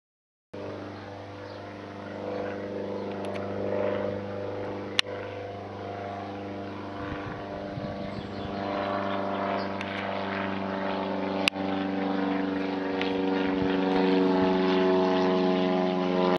Water Lane Flyover
This plane as it flew over Water Lane here in Smarden when I was walking around the village.